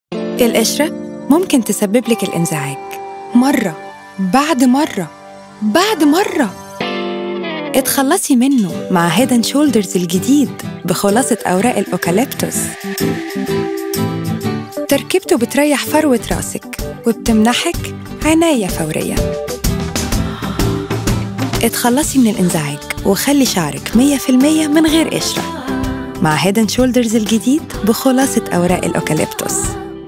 New H&S Itch Free- هيد اند شولدرز الجديد ضد الحكّة
القشرة ممكن ان تسبب الحكة .
تخلصي منه مع هيد اند شولدرز الجديد بخلاصة اوراق آوكاليبتوس